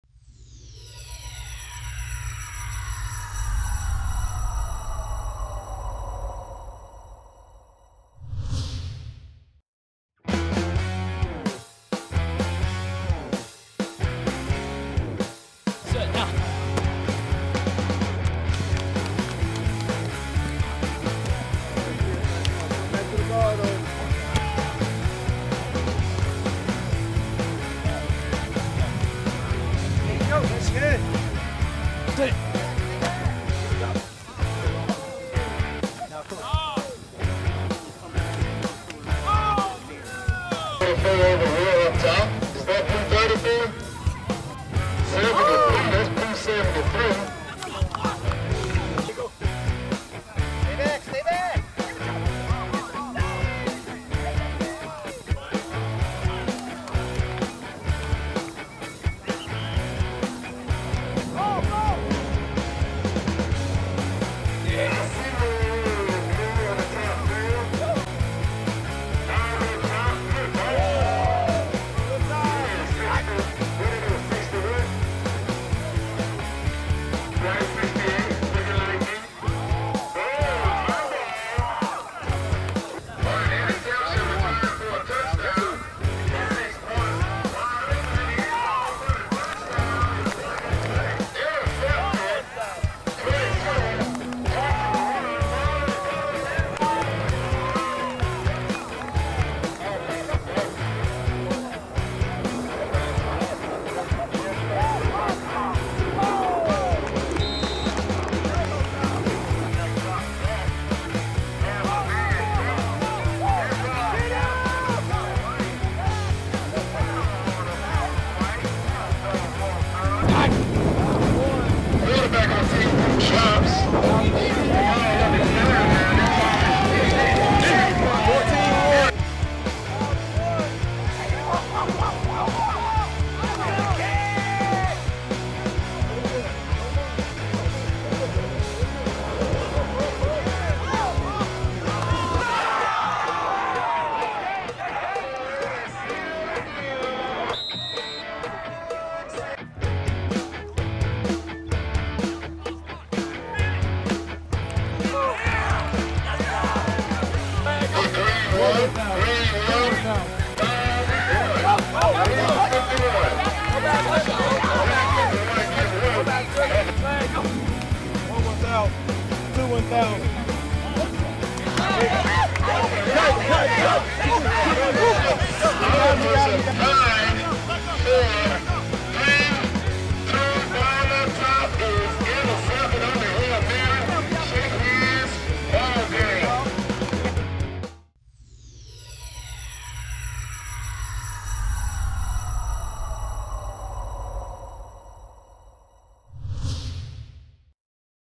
NUC - U100 Southeast 2012 - 11th Grade - Skills Players Event Highlights - National Underclassmen
Watch these highlights from the 11th GradeSkills Players 1 on 1s and 7 on 7 Tournament at the U100 Southeast 2012 combine by National Underclassmen.

Source: Royaltyfreemusic.com